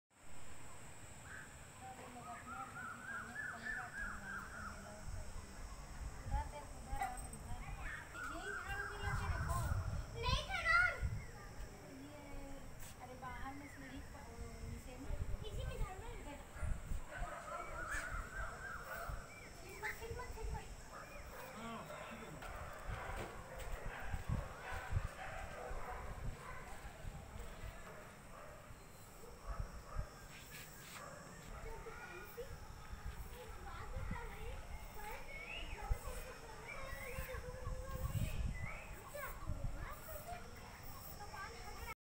cloudy sky || birds go back to their home || good evening nature
In this video, I am showing birds, Cloudy Sky, Evening time and Birds go back their home nest